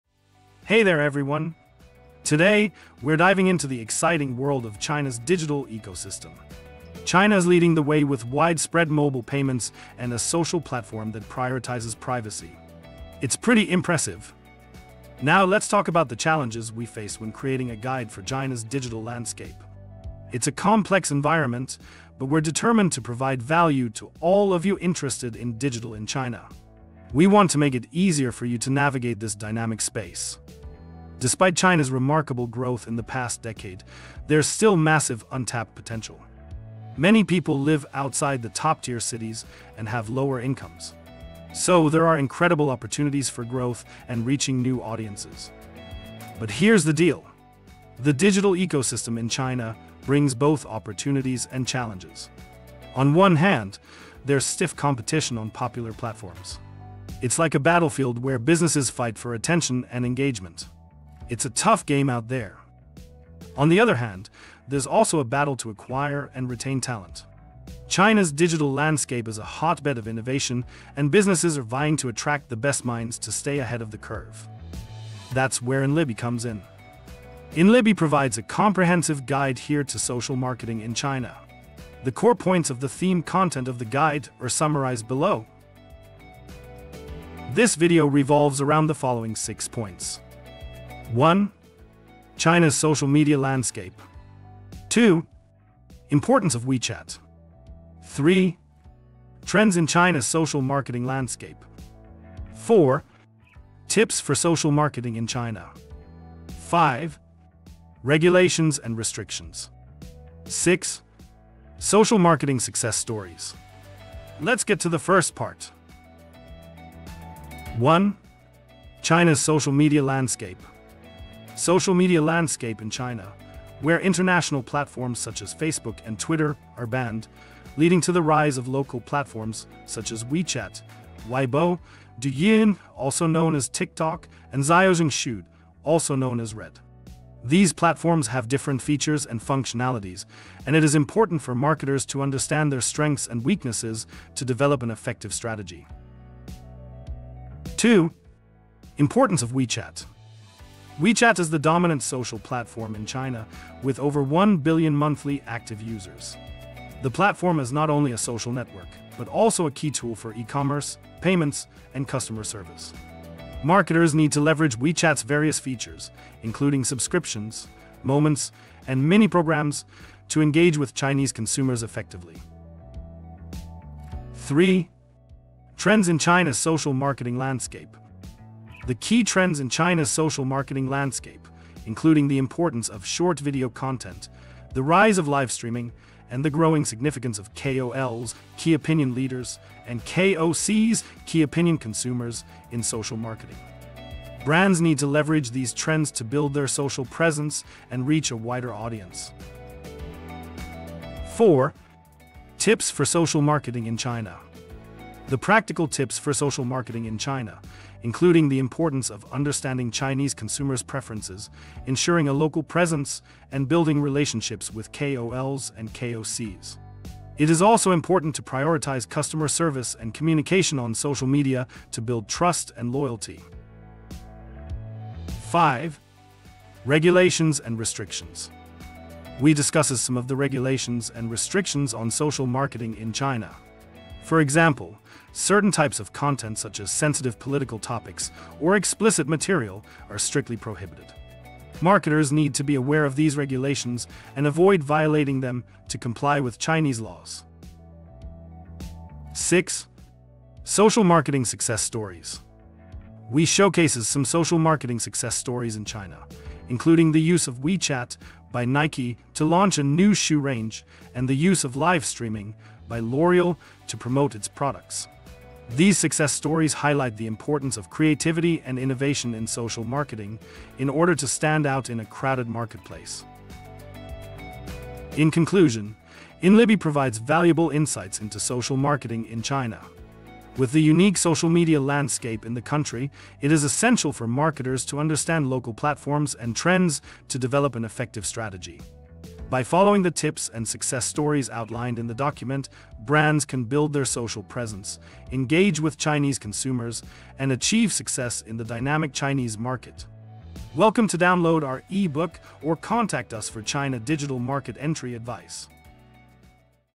China Digital 101: Mastering Top Social Media in China's Digital Landscape
Welcome to our YouTube video, "China Digital 101: Mastering Social Media in China's Digital Landscape." In this comprehensive guide, we're going to unlock the secrets of thriving in China's dynamic digital world.

China is a powerhouse in the digital ecosystem, and understanding its unique landscape is crucial for success. From widespread mobile payments to a privacy-focused social platform, China is at the forefront of innovation. However, navigating this intricate digital landscape can be challenging.

That's where our guide comes in. We've done the research, gathered the insights, and crafted a roadmap to help you navigate the complexities of social media in China. Whether you're a marketer, business owner, or simply curious about the digital trends in China, this video is for you.

Get more similar resources in one go from here👇👇👇🏆🏆🏆